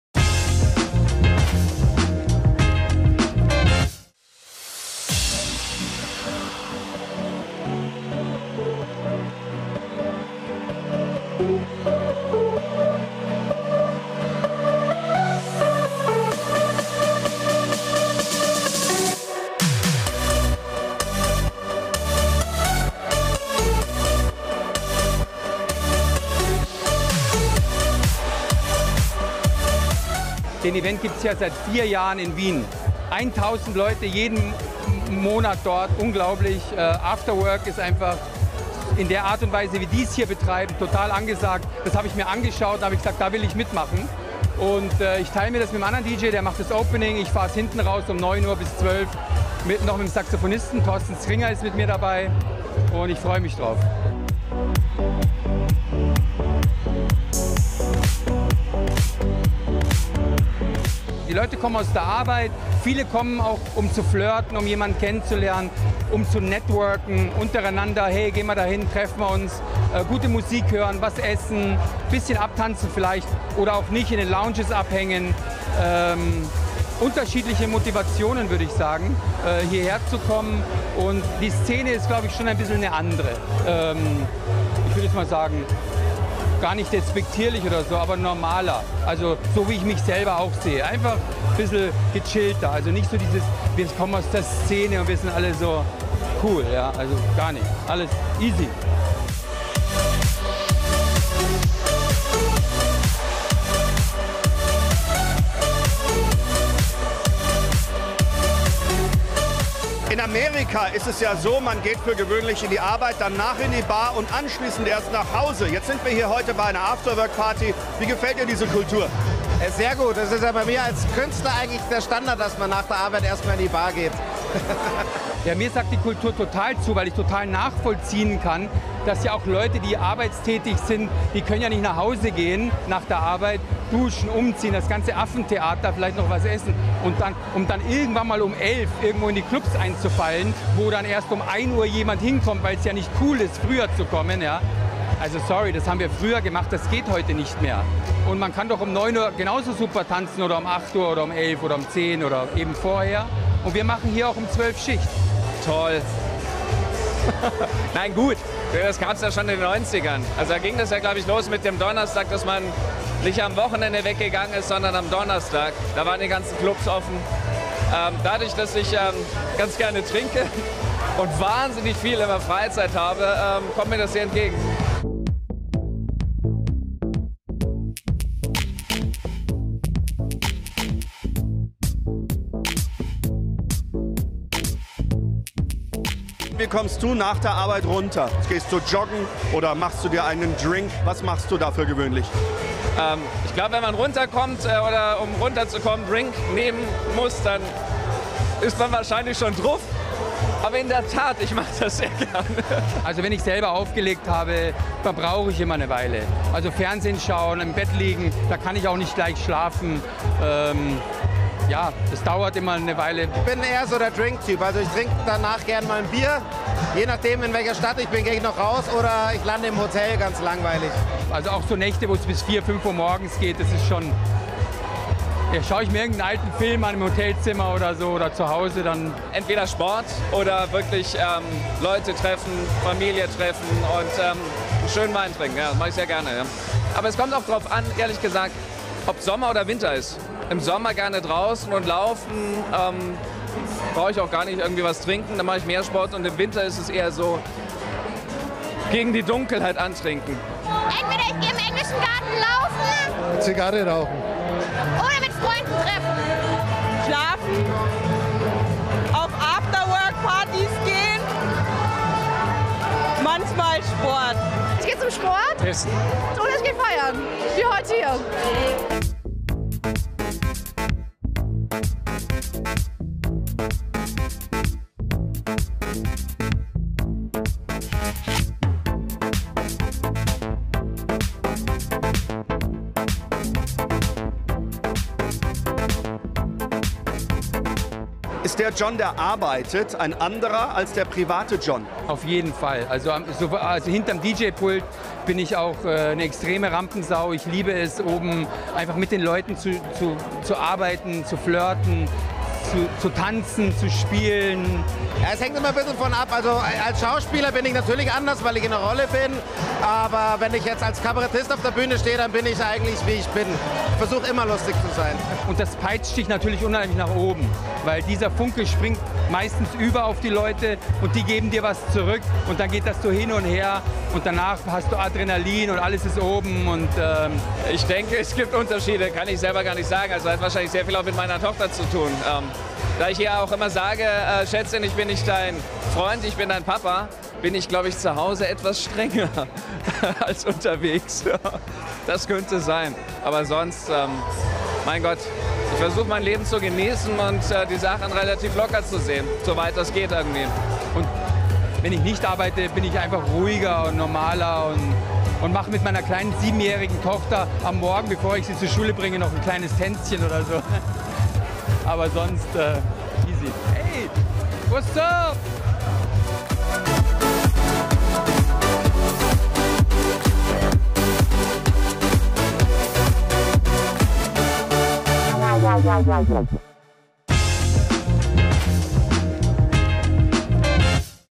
Büroschluss! Münchens neue After Work Party
Von der Arbeit direkt auf die Tanzfläche: Mit "Büroschluss" ist eine neue "After Work Party"-Reihe in München gestartet. Dort haben wir u.a. John Jürgens (Dj John Munich) getroffen.…